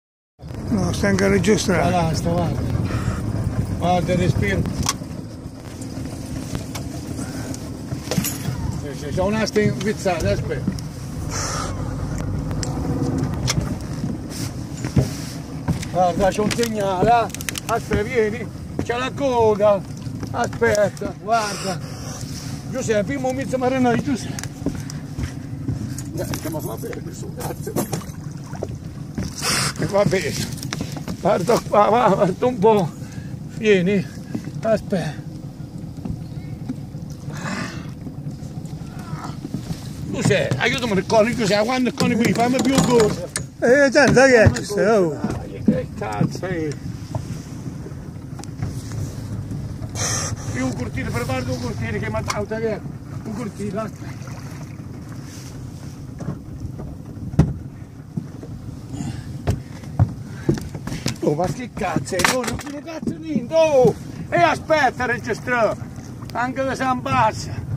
Salvataggio delfino impigliato
Rischiava di morire affogato se non fossimo intervenuti a tagliare la cima che lo aveva bloccato sulla coda...